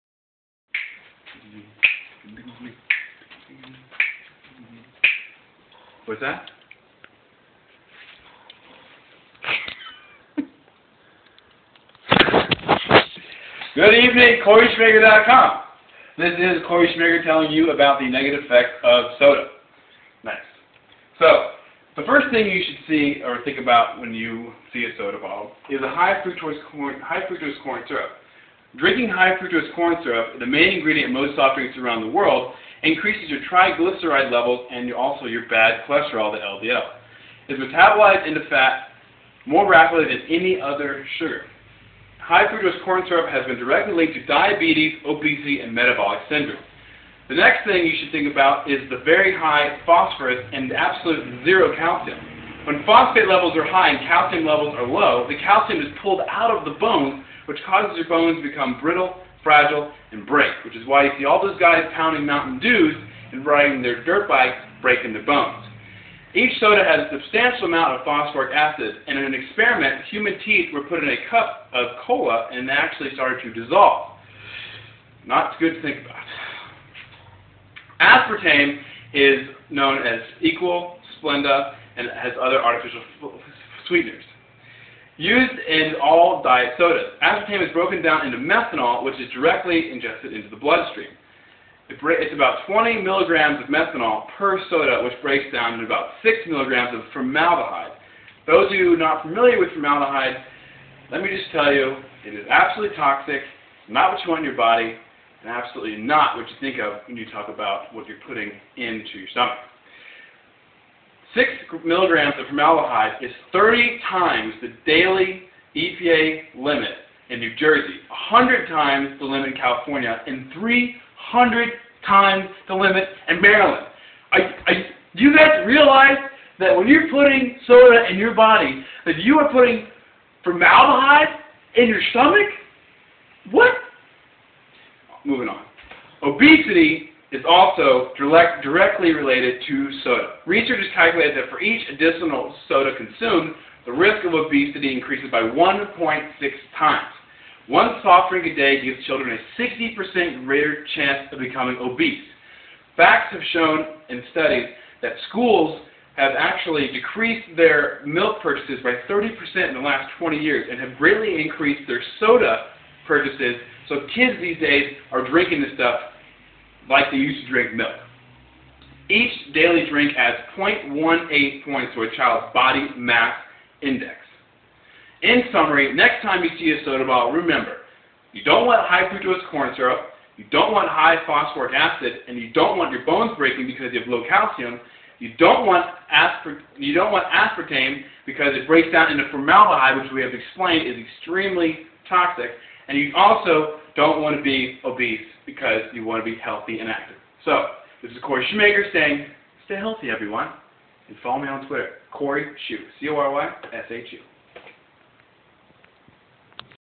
Negative Effects of Soda
High Fructose Corn Syrup
• "Drinking high-fructose corn syrup (HFCS), the main ingredient in most soft drinks throughout the world, increases your triglyceride levels and your LDL (bad) cholesterol."
• "...it is metabolized to fat in your body far more rapidly than any other sugar"
• HFCS has been directly linked to Diabetes, Obesity and Metabolic Syndrome.
Very High Phosphorus & Zero Calcium
• "When phosphate levels are high and calcium levels are low, calcium is pulled out of the bones."
• Each soda has substantial amounts of phosphoric acid; in an experiment, human teeth began to dissolve when put in a cola beverage.
• Acidity of soda is the same as vinegar.
• "Tooth loss and gingivitis can be problems, especially with a high phosphorus intake"
• "Phosphoric acid infiltrates bodily fluids and corrodes stomach linings, it upsets the alkaline-acid balance of the kidneys, and eats away at your liver"
Aspartame
• AKA: Equal, Splenda, and other artificial sweeteners.
• Used as a sweetener in all diet sodas.
• The aspartame is broken down into methanol when ingested and goes into the blood stream.
• "Each diet soda with aspartame produces about 20 mg of methanol. The methanol breaks down further into 6 mg of formaldehyde which is three times the daily EPA limit. It's 30 times the limit in New Jersey, 100 times the limit in California, and 300 times the limit in Maryland."
Obesity
• "...researchers calculate that for each additional soda consumed, the risk of obesity increases 1.6 times."
• "One soft drink a day gives children a 60% greater chance of becoming obese."
• "Each daily drink added .18 points to a child's body mass index (BMI)."
Summary
• High in phosphoric acid, low in calcium
• Aspartame breaks down into formaldehyde
• Obesity